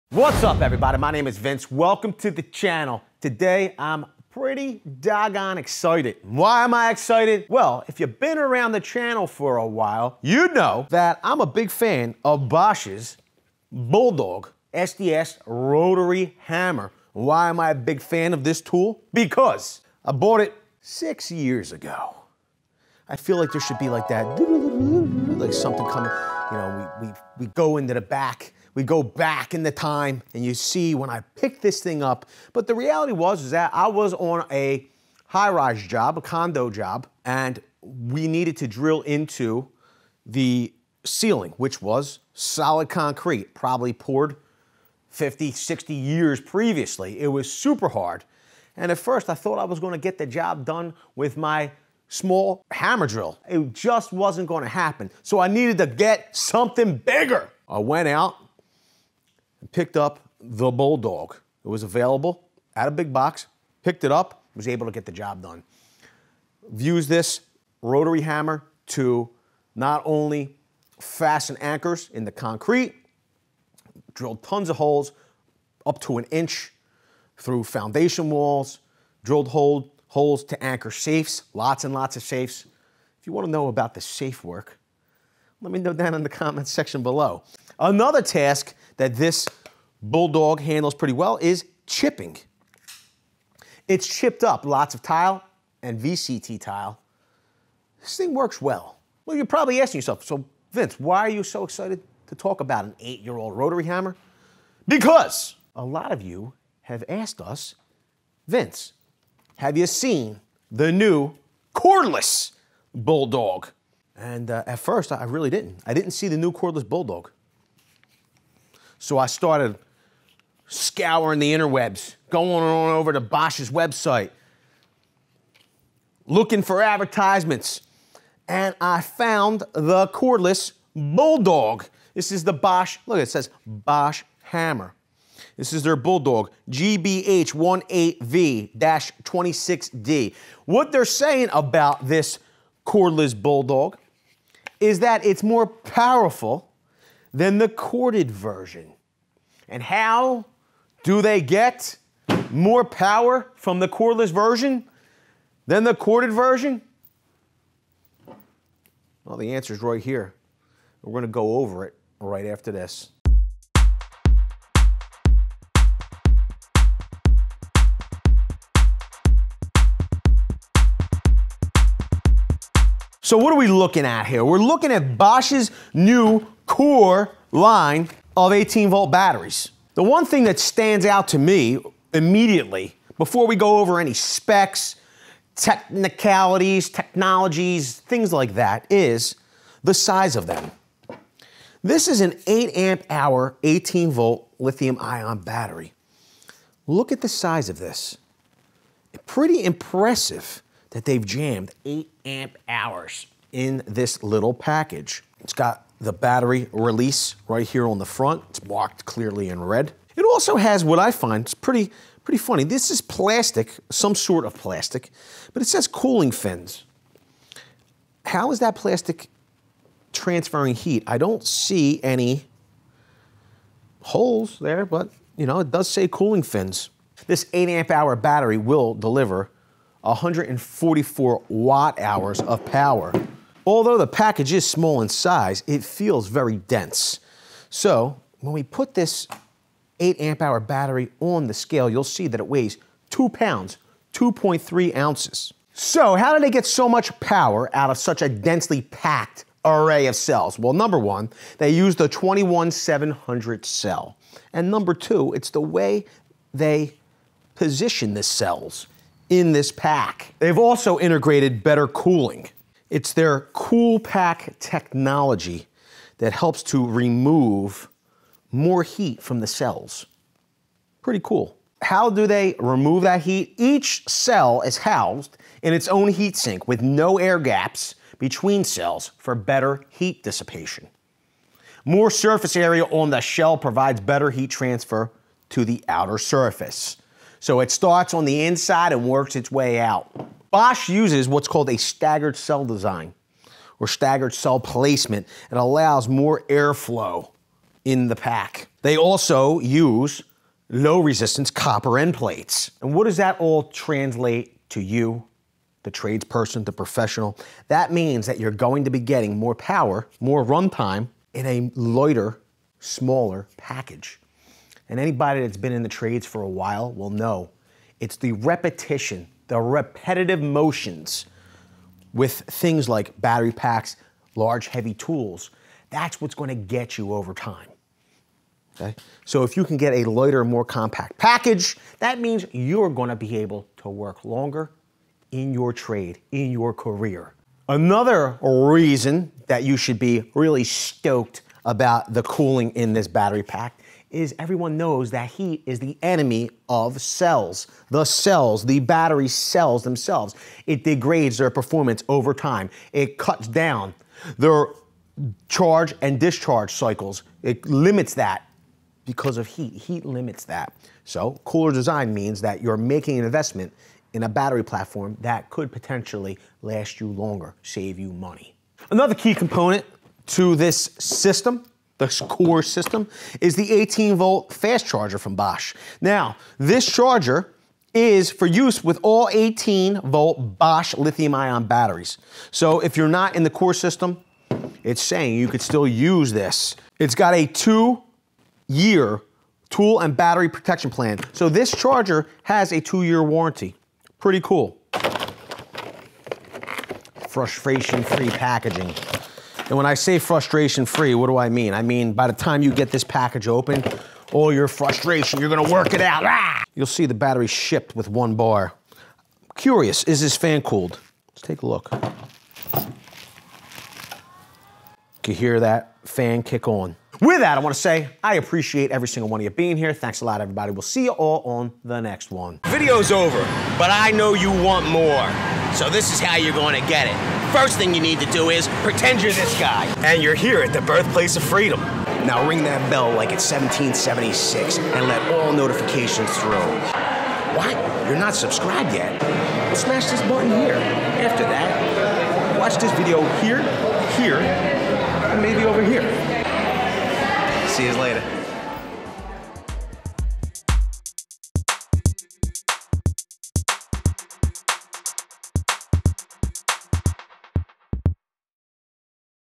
What's REALLY Going On With BOSCH 18V CORE Batteries (bet you didn't know this)
Bosch CORE 18V batteries have some really cool features built into them and today we are giving you the rundown! Did you know about the features of these Bosch CORE 18V batteries? Or did you learn something new?